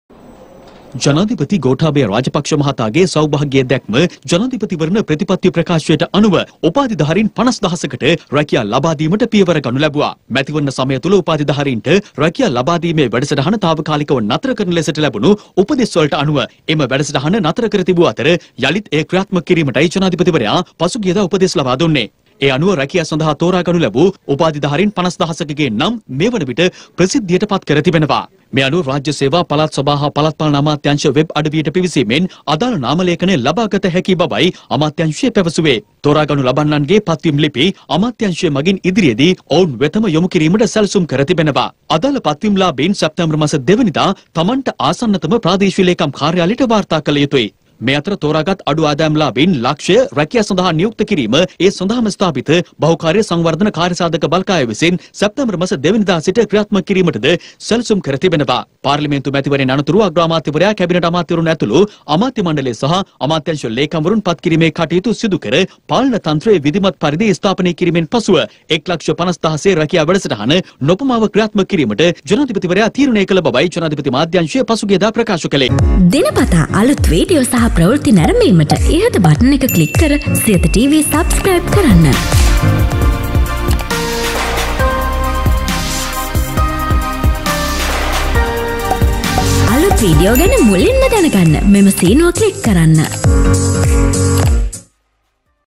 රැකියාලාභී උපාධිධාරීන් 50000 කගේ නම් ප්‍රසිද්ධ කෙරේ | Siyatha News
රැකියාලාභී උපාධිධාරීන් 50000 කගේ නම් ප්‍රසිද්ධ  කෙරේ